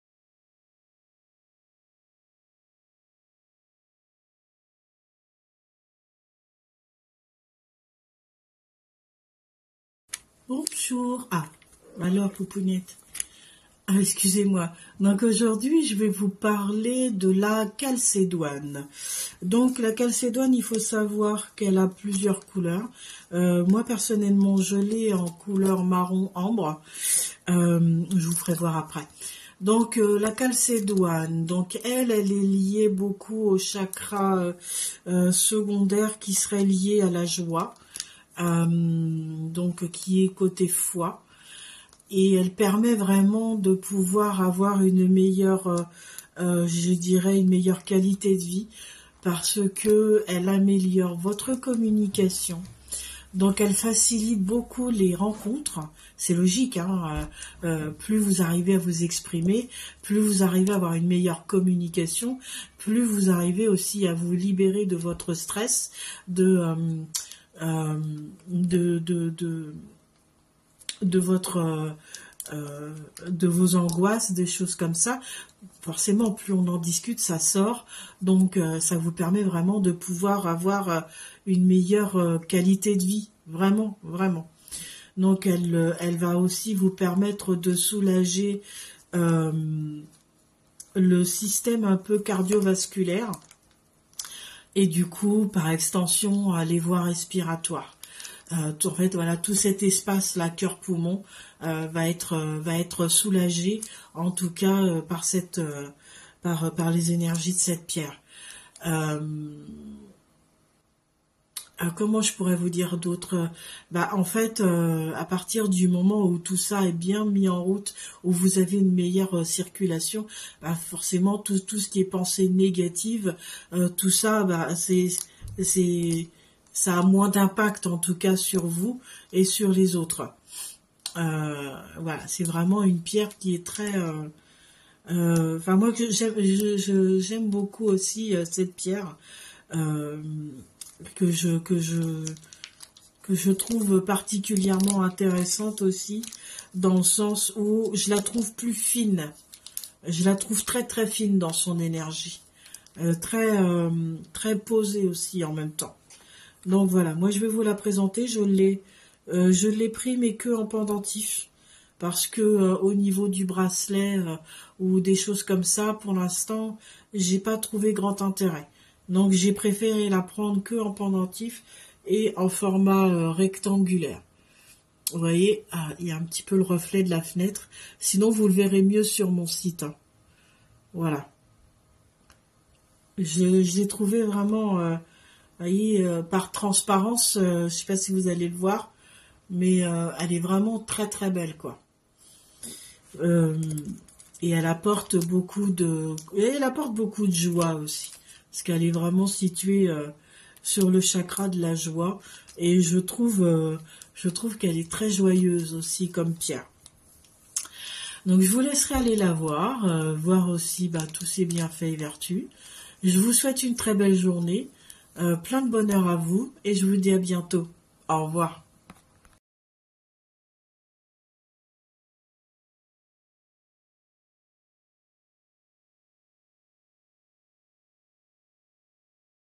Calcédoine
Lithothérapie

Vertus et bienfaits de la Calcédoine
- Améliore la communication orale
- Facilite les rencontres
- Soulage et guérit les voies respiratoires
- Diminue le bégaiement et troubles du langage
- Protège le système cardio-vasculaire
- Lutte contre la rétention d’eau
- Favorise l’allaitement
- Abaisse le stress et l’anxiété
- Aide à prendre confiance en soi
- Chasse les pensées négatives